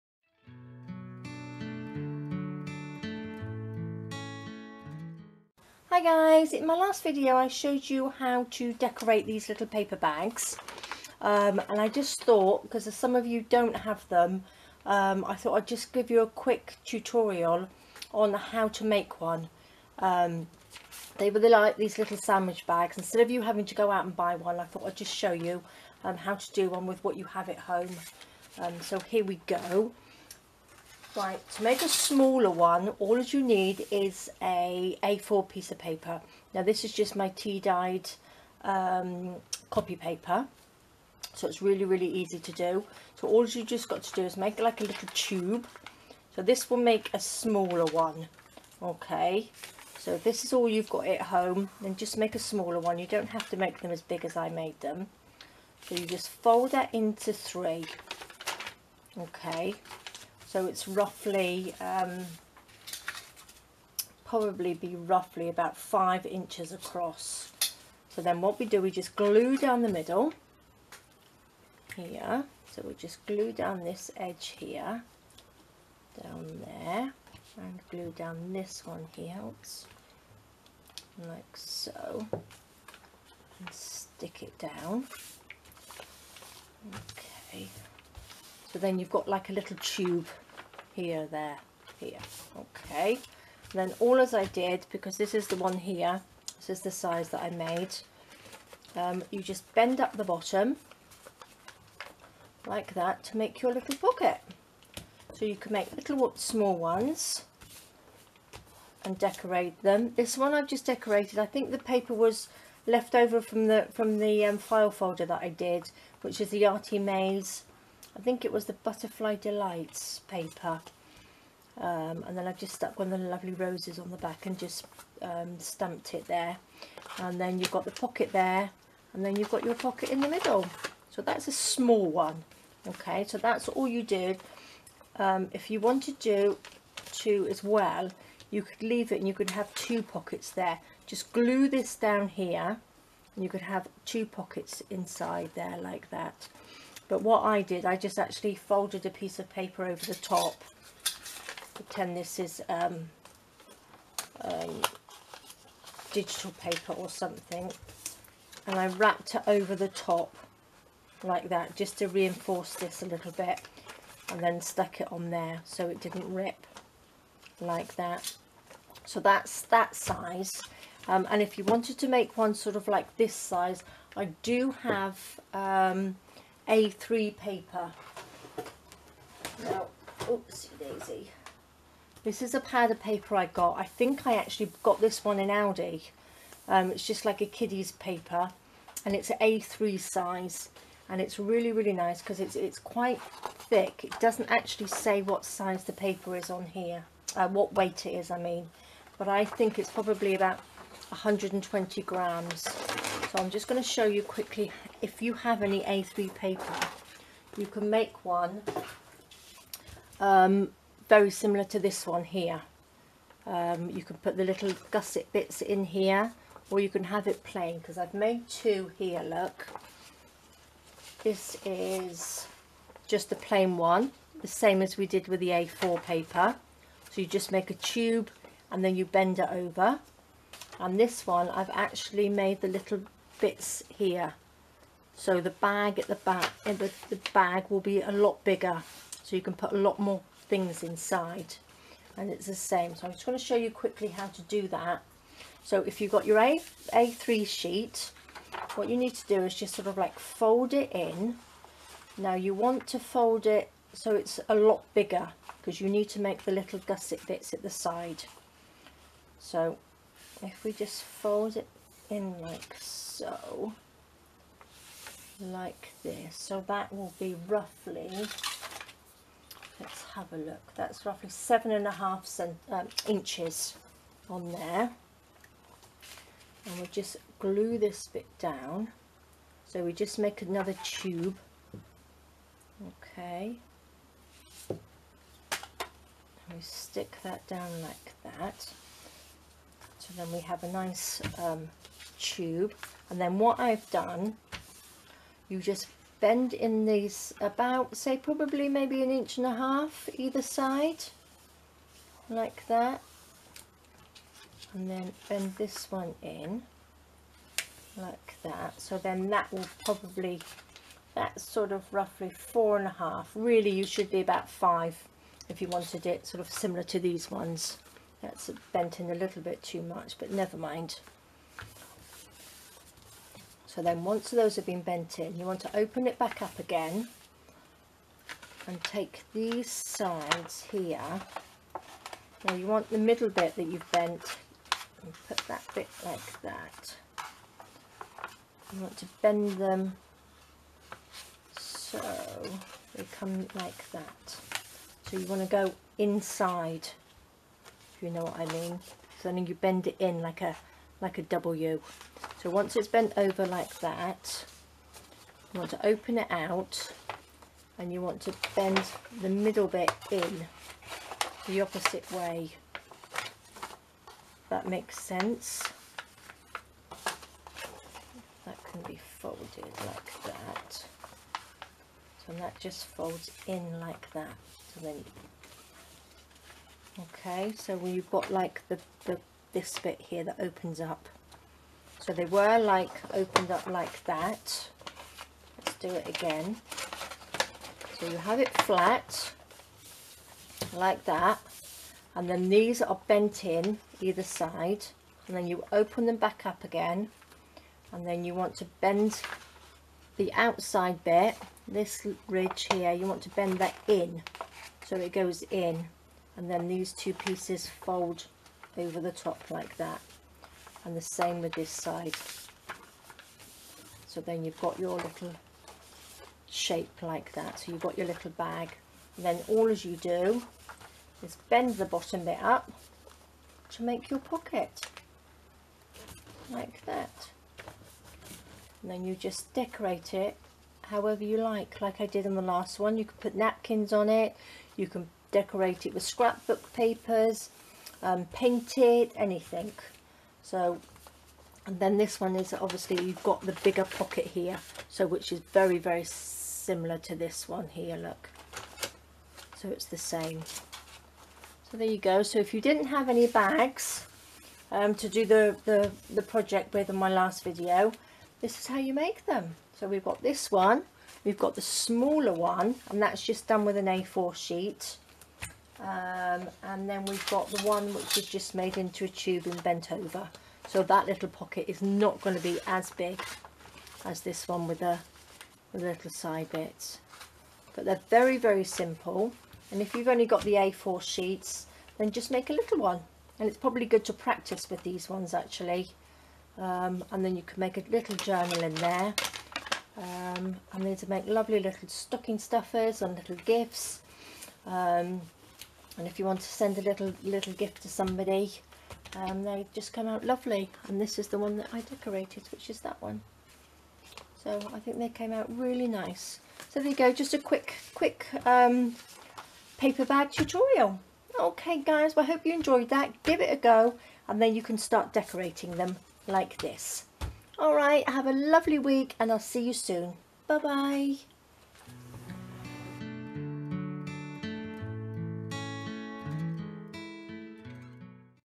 Craft Along With Me Making a Quick Paper Bag to Decorate
I should of made this video before the video where I decorated a paper bag! Never mind!
This is just a short video to show you how to make a quick and easy paper bag to decorate like the one in this video.

This is my other passion, painting Dragon eyes, Please take a look at my Facebook page.

Thank you X Love and Hugs Gi X